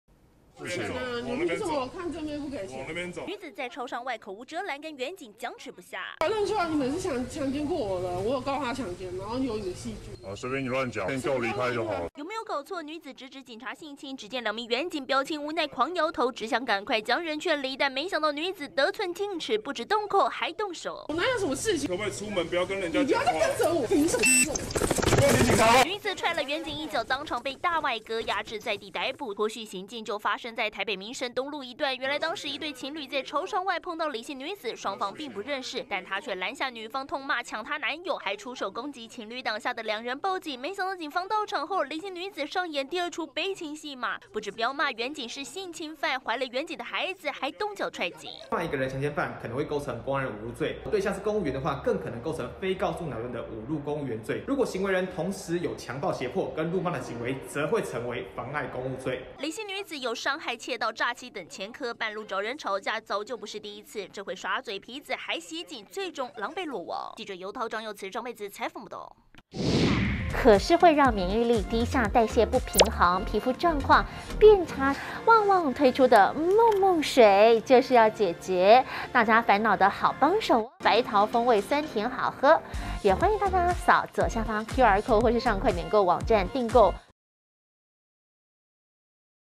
【每日必看】半路攻擊情侶檔! 脫序女反控員警性侵 20230306@中天新聞CtiNews
台北市一名女子日前在超商外遇到陌生情侶檔，竟莫名指控女方搶她男友，還動手攻擊情侶，嚇得兩人報警處理。警方到場後，女子竟反指兩名員警是性侵犯，還出腳狂踹警察，最後遭壓制在地，被依妨害公務罪送辦。